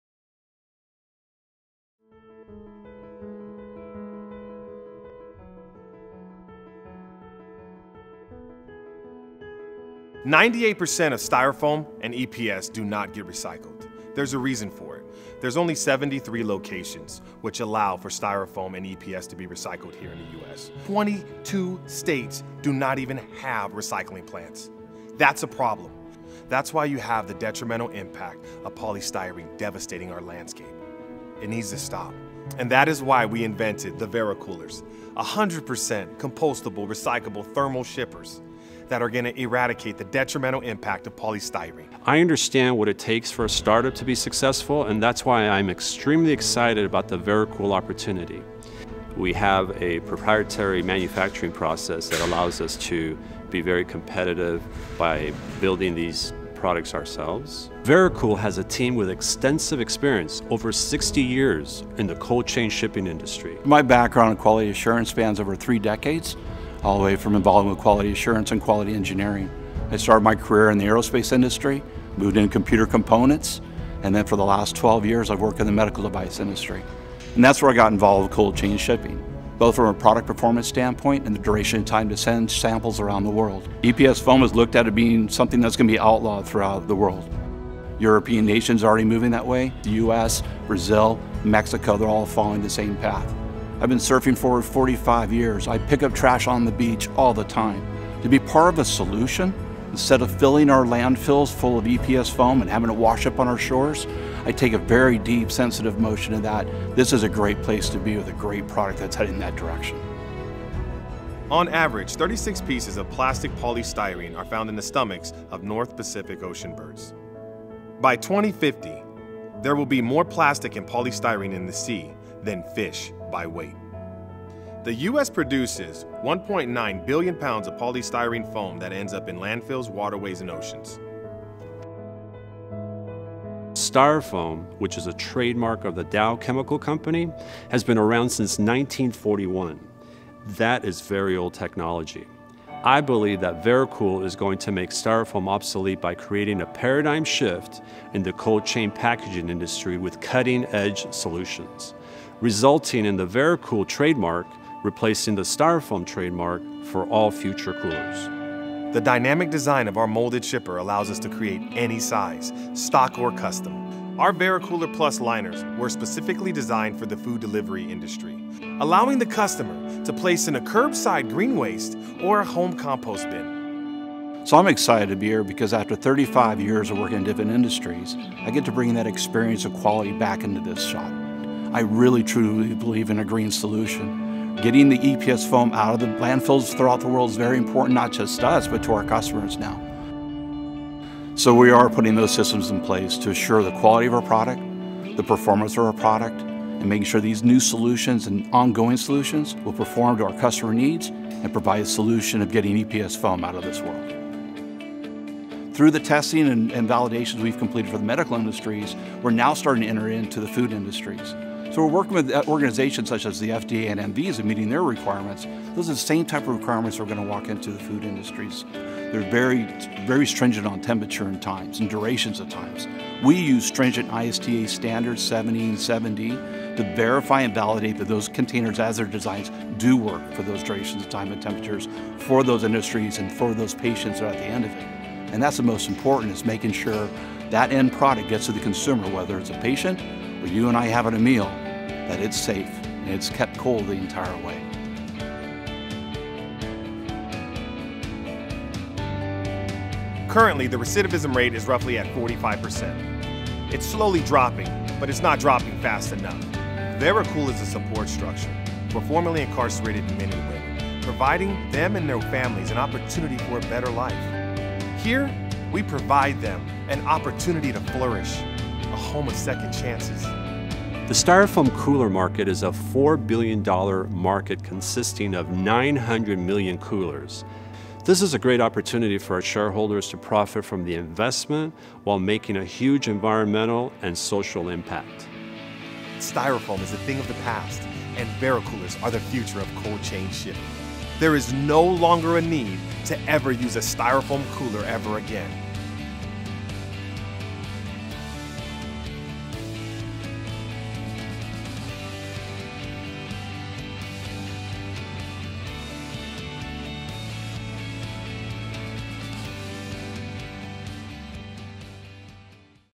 Vericool Shifting Market with 100% Compostable/Recyclable Thermal Styrofoam Cooler Replacements
A compostable & recyclable cold shipping solution that’s as affordable, and can out-perform molded EPS (Styrofoam)! Designed for the Bio-Tech, Medical, Food delivery, specialty Pharma and Health industries.